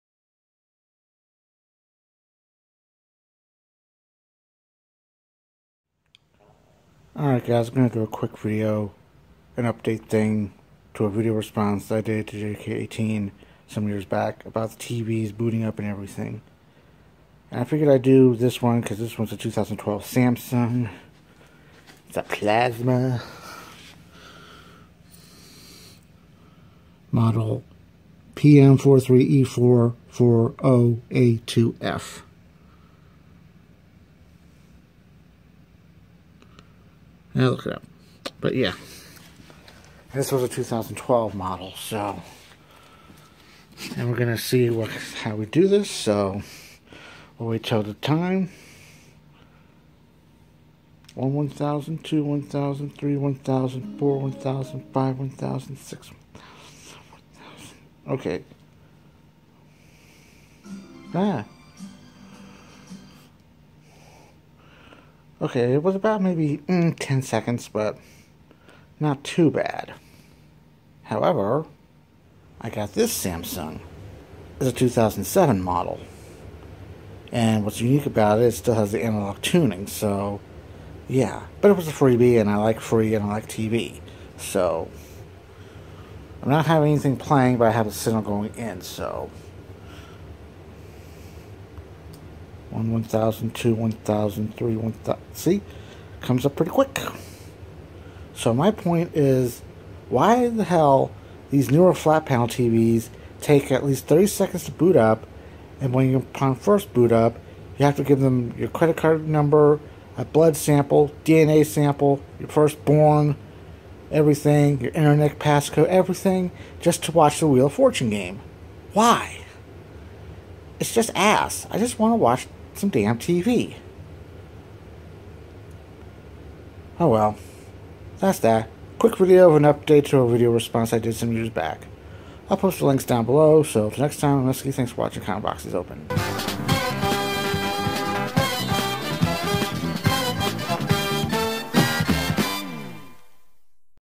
Quick Video: Older Flat Panels Boot Faster
An update video to a video response to a video @jaykay18 made around 2016 where I missed the game since my TV took long to boot up ( or something like that).

The two television monitors are both by Samsung. The big TV is a plasma display model PN43E440A2F form 2012. The second one was a freebie and it's a model LN-R2050P from 2007 (made Octember 2006).

Point is why do older flat panel TVs come up quick, but ones made six months ago take 30 seconds, and require other things??